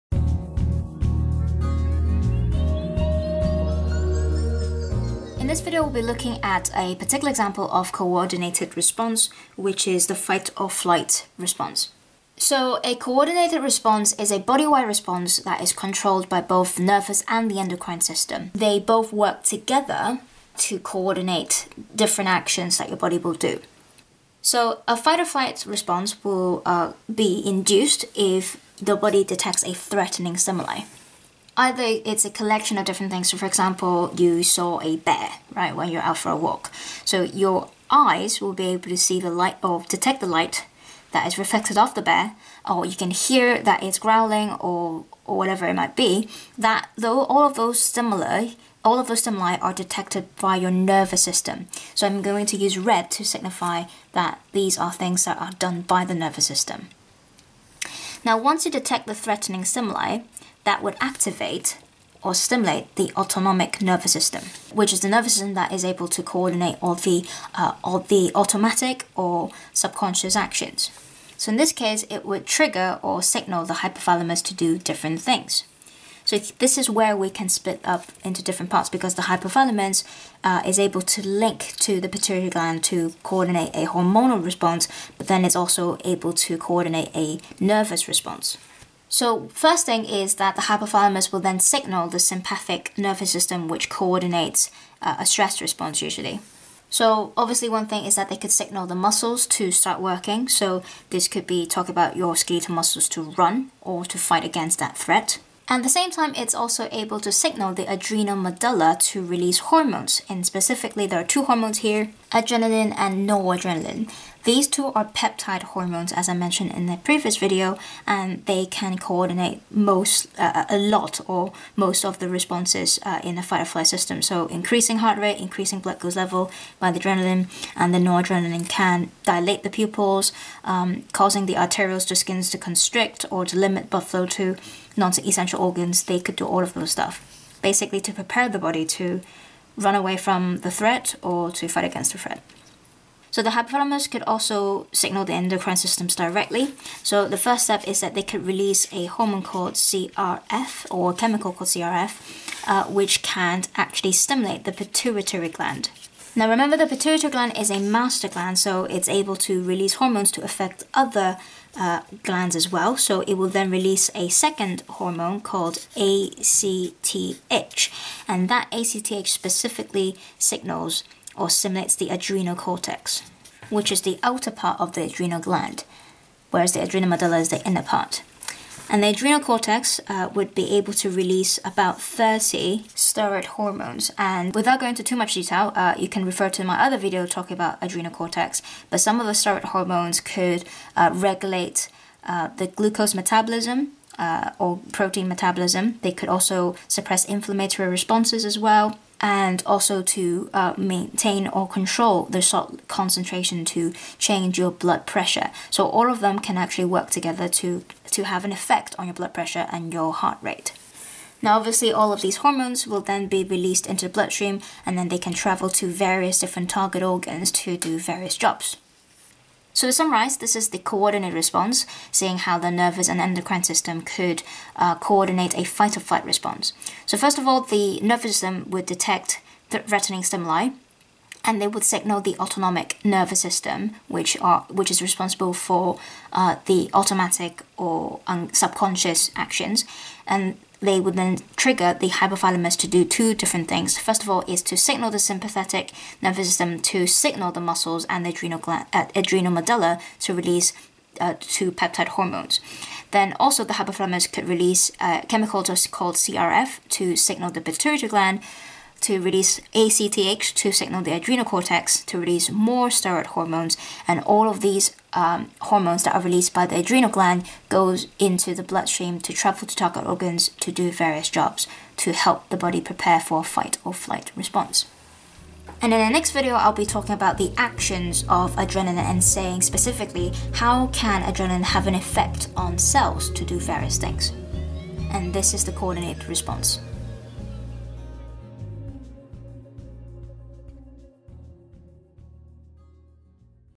A2 Biology - Coordinating stress response (OCR A Chapter 14.5)
This video outlines the events coordinated by both the nervous and endocrine systems to respond to a stress signal.

If you liked my videos and would like to support me, then I'd really appreciate you buying me a cup of tea or a piece of cake! :D